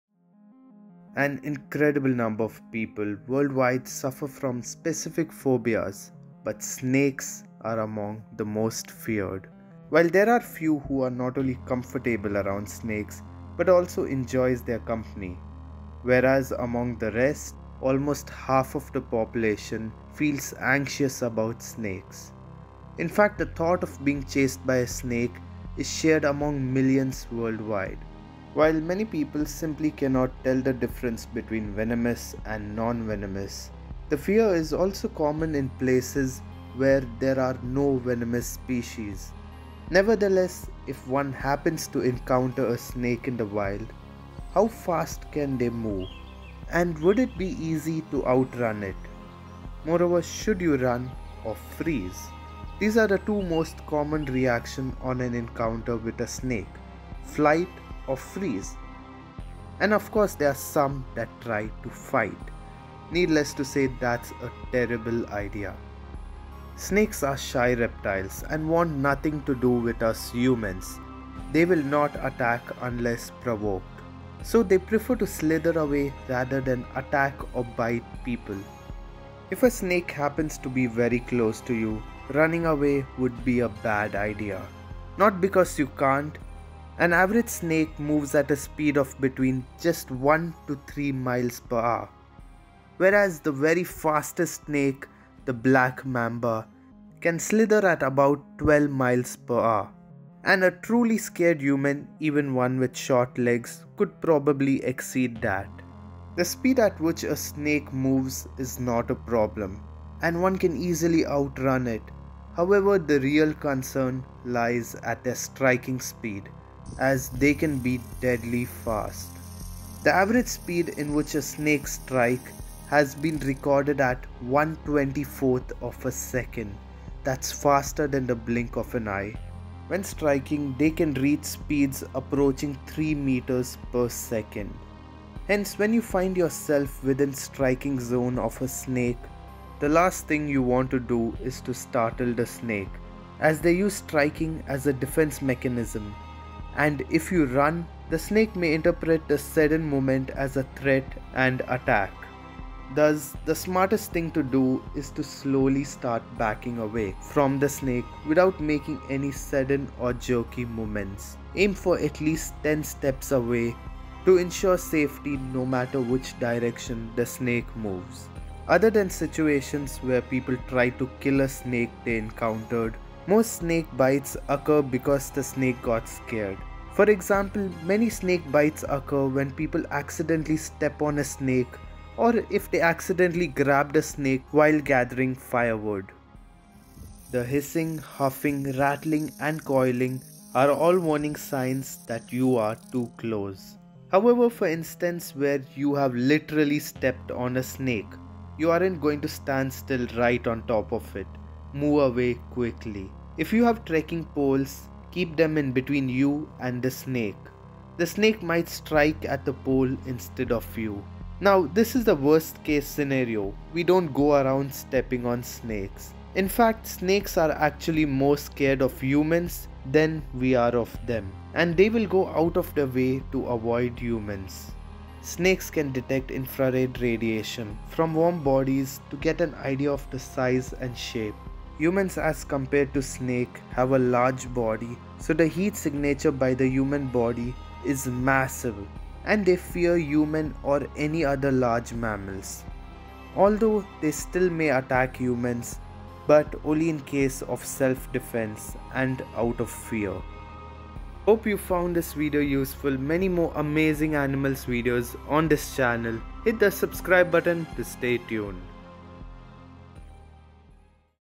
How Fast Can Snakes Move?
If you have ever wondered How fast can a snake move? and if you can outrun a snake?
Here is the answer to your question! In this video we speak about the behaviour of a snake and how could one possibly avoid an attack from a snake!

This video contains information on how to avoid snake bite / how to avoid attack from snakes, We discuss how fast does a snake move ( snake speed ) and how fast does snakes strike ( snake striking speed ) and wether you could outrun a run? moreover should you run away from a snake?

This video covers the subject of what to do when you see a snake?

An incredible number of people worldwide suffer from specific phobias but snakes are among the most feared.

While there are few who are not only comfortable around snakes but also enjoys their company whereas among the rest

Almost half of the population feel anxious about snakes.

Infact the thought of being chased by a snake is shared among millions worldwide.

While many people simply cannot tell the difference between venomous and non-venomous, the fear is also very common in places where there are no venomous species.

Nevertheless if one happens to encounter a snake in the wild, how fast can they move and would it be easy to outrun it?

Moreover should you run or freeze?

These are the two most common reactions on an encounter with a snake - flight or freeze.

And ofcourse there are some that try to fight, needless to say that's a terrible idea.

Snakes are shy reptiles and wants nothing to do with us humans. They will not attack unless provoked, so they prefer to slither away rather than attack or bite people.

If a snake happens to be very close to you, running away would be a bad idea not because you can't.

An average snake moves at a speed of between just 1 to 3 MPH, whereas the very fastest snake, the Black Mamba, can slither at about 12 MPH, and a truly scared human (even one with short legs) could probably exceed that.

The speed at which a snake moves is not a problem and one can easily outrun it however the real concern lies at their striking speed as they can be deadly fast.

The average speed in which a snake strikes has been recorded at 1/24th of a second, That's faster than the blink of an eye.

When striking they can reach speeds approaching 3 metres per second.

Hence when you find yourself within striking zone of a snake, running would be a bad idea.

The last thing you want to do is to startle the snake as they use striking as a defence mechanism and If you run, the snake may interpret the sudden movement as a threat and attack.

Thus, the smartest thing to do is to slowly start backing away from the snake without making any sudden or jerky movements.

aim for at least 10 steps away, to ensure safety no matter which direction the snake moves.

Other than situations where people try to kill a snake they encountered, most snake bites occur because the snake got scared.  For example, many bites occur when people accidentally step on a snake or if they accidentally grabbed a snake while gathering firewood.

The Hissing, huffing, rattling, and coiling, are all warning signs that you are too close.”

However for instance where you have literally stepped on a snake or right next to it, you aren’t going to stand still right on top of it.  Move away quickly.  If you have trekking poles, keep them in between you and the snake — the snake might strike at the poles instead of you.

Now this is worst case scenario, We don't go around stepping on snakes.

Infact Snakes are actually more scared of humans than we are of them

And they will go out of their way to avoid humans.

Snakes can detect infrared radiation from warm bodies to get an idea of the size and shape.

Humans as compare to snake have a large body so the heat signature by the human body is massive and they fear human or any other large mammals

Although they still may attack humans but only in case of self defense out of fear.

Hope you found this video useful,
Many more amazing animals videos on this channel